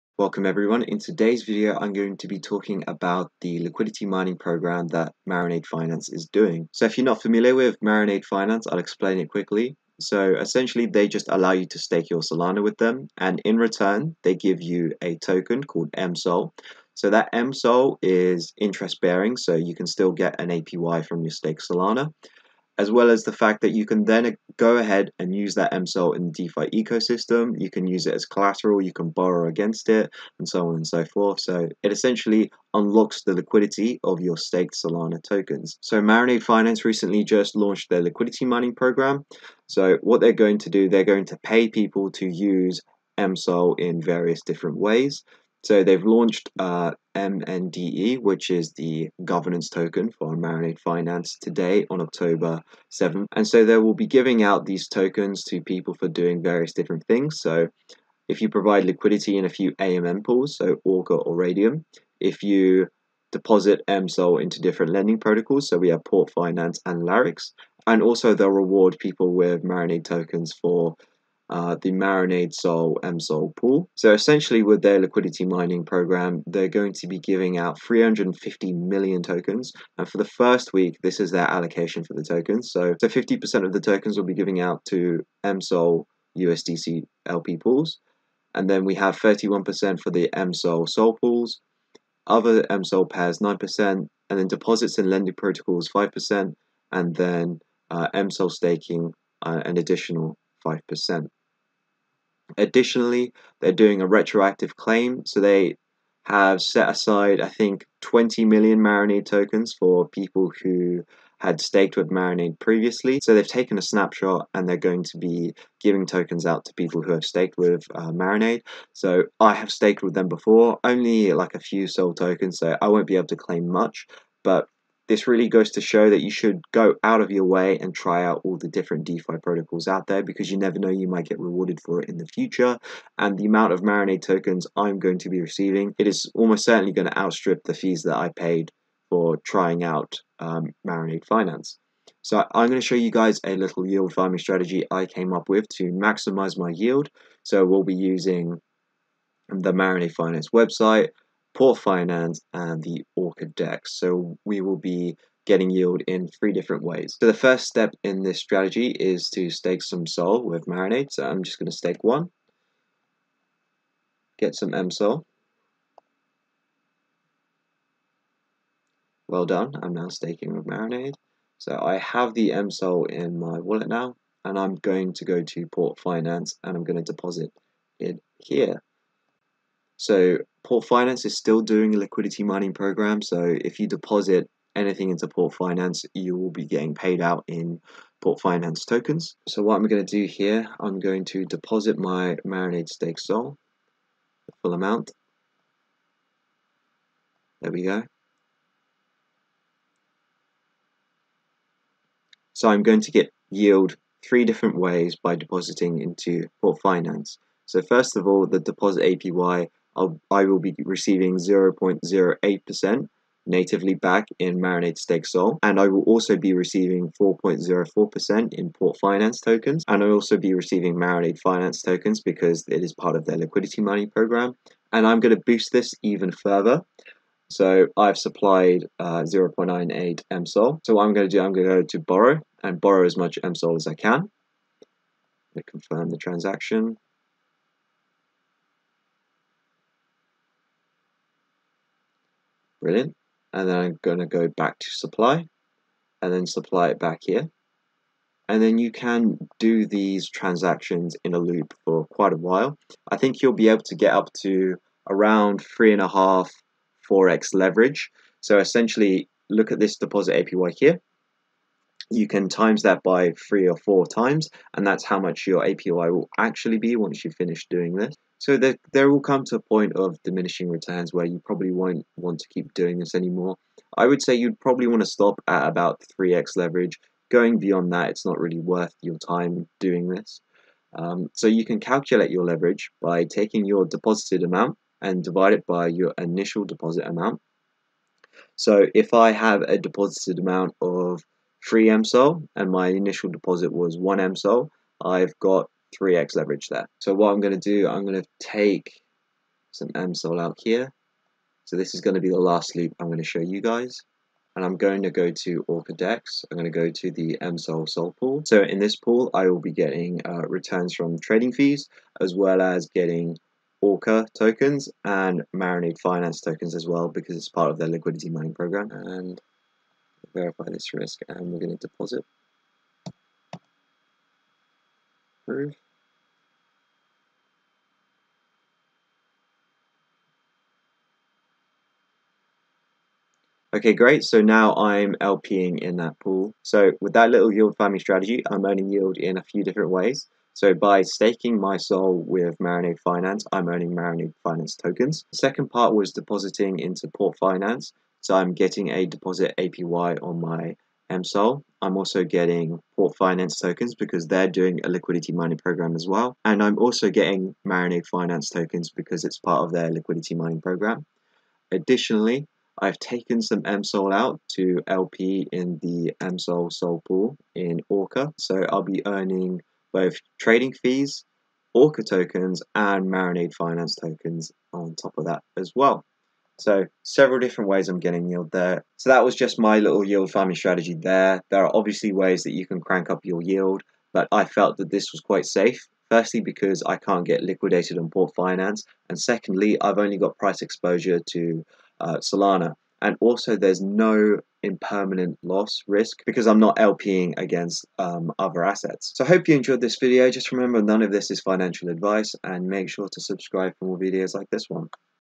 Get Paid To Use Marinade Finance l Marinade x Port Liquidity Mining
In this video I show you guys the current liquidity mining programme going on right now with Marinade Finance. I also go into a yield farming strategy that uses Marinade Finance, Port Finance and Orca Dex.